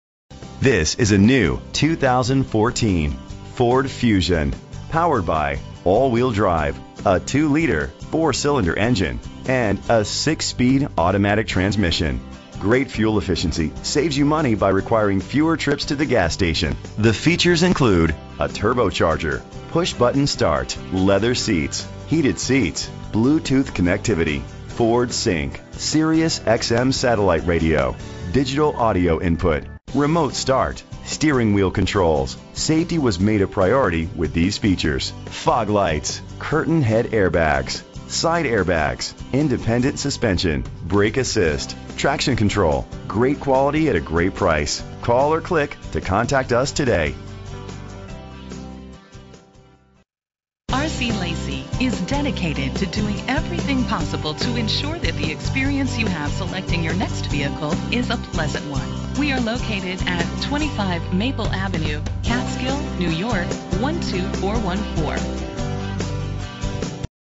2014 Ford Fusion - CATSKILL NY
Phone:
Year: 2014
Make: Ford
Model: Fusion
Trim: Titanium
Engine: 2.0 liter inline 4 cylinder 16 valve
Transmission: 6-Speed Automatic
Color: Ingot Silver Metallic
Mileage: 5
Address:
25 HIGH ST